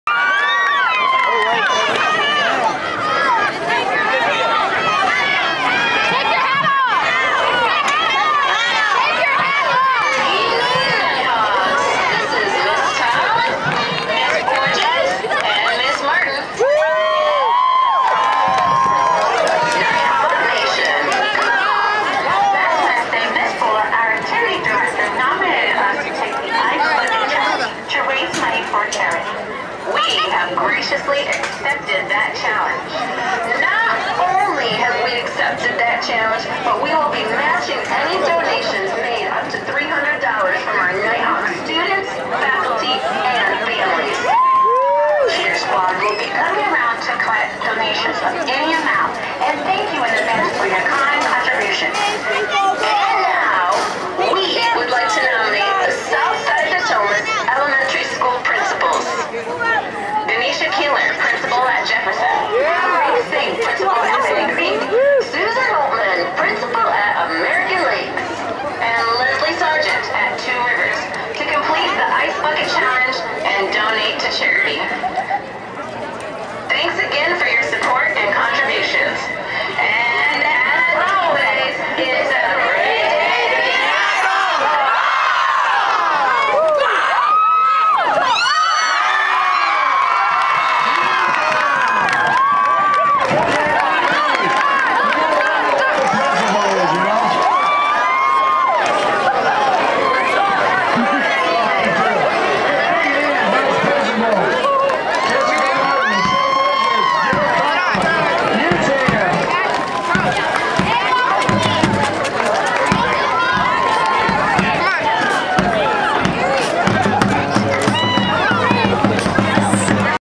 NHS Vice Principal Ice Bucket Challenge
NHS vice principals Jill Thom, Shea Borges, and Jessica Martin step up for the ice bucket challenge and now have challenged Danisha Keeler, Susan Holtemann, Amreek Singh, and Leslie Sargent